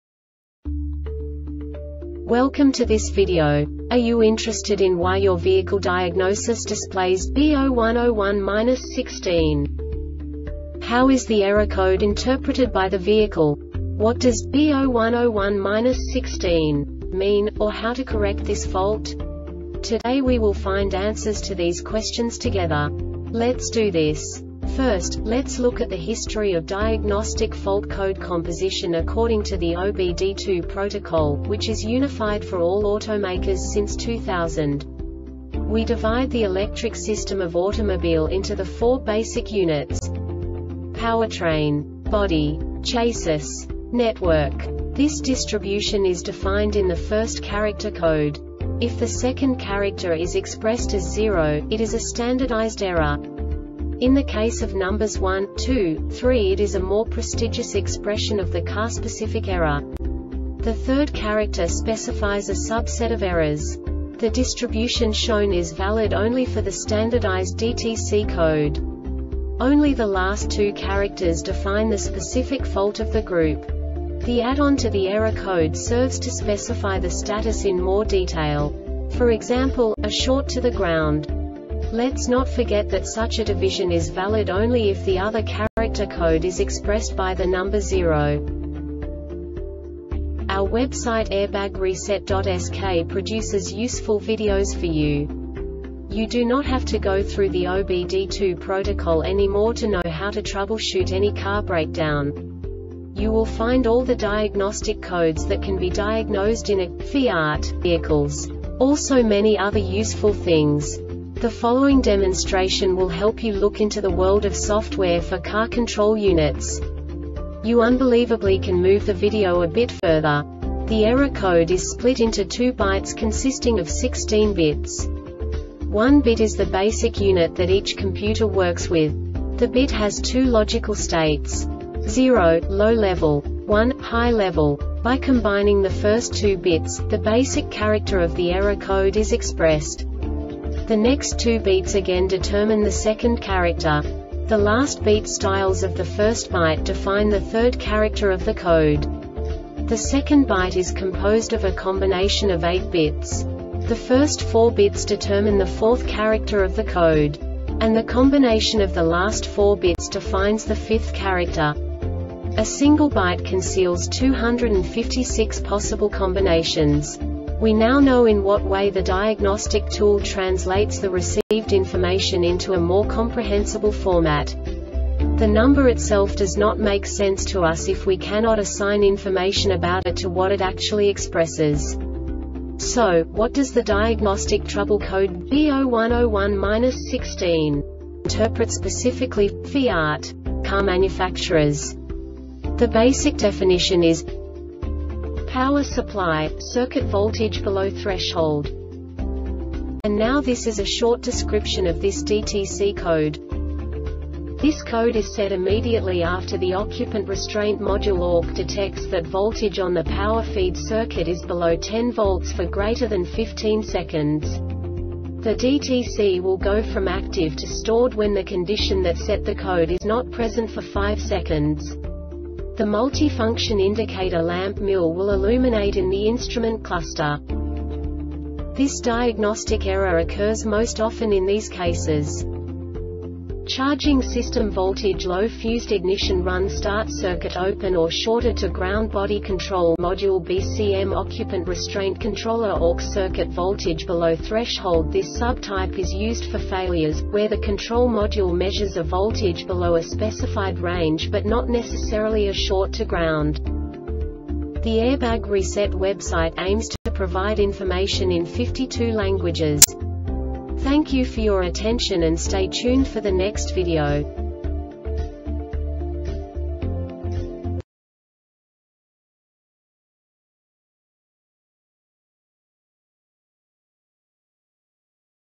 DTC Fiat B0101-16 Short Explanation
Contents:
0:21 Basic DTC analysis according to OBD2 protocol standard.
1:48 Insight into programming
2:58 A diagnostically understandable description of the Diagnostic Trouble Code
3:05 Basic error definition

Make:
FIAT
Code:
B0101-16
Definition:
Power Supply - Circuit Voltage Below Threshold
Description:
This code is set immediately after the Occupant Restraint Module (ORC) detects that voltage on the power feed circuit is below 10 volts for greater than 15 seconds. The DTC will go from active to stored when the condition that set the code is not present for five seconds. The Multifunction Indicator Lamp (MIL) will illuminate in the Instrument Cluster.
Cause:
Charging System Voltage Low
Fused Ignition Run or Start Circuit Open Or Shorted To Ground
Body Control Module (Bcm)
Occupant Restraint Controller (Orc)
Failure Type: